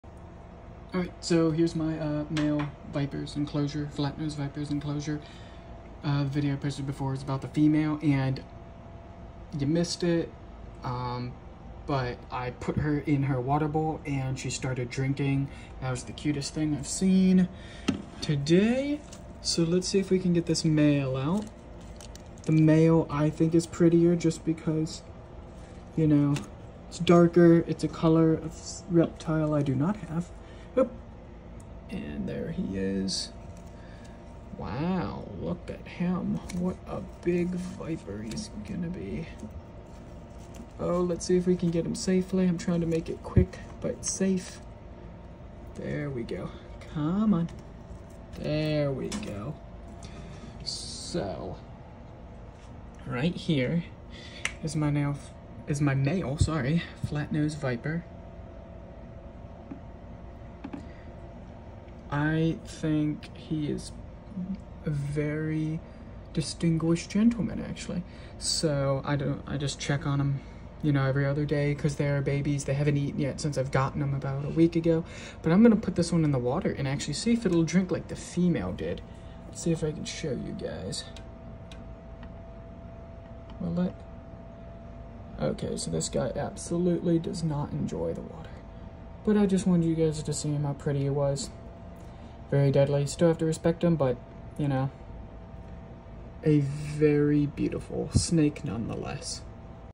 He’s an amazing viper
Craspedocephalus puniceus is a venomous pit viper species endemic to Southeast Asia. Common names include: flat-nosed pitviper, flat-nosed pit viper, and ashy pit viper. No subspecies are currently recognized.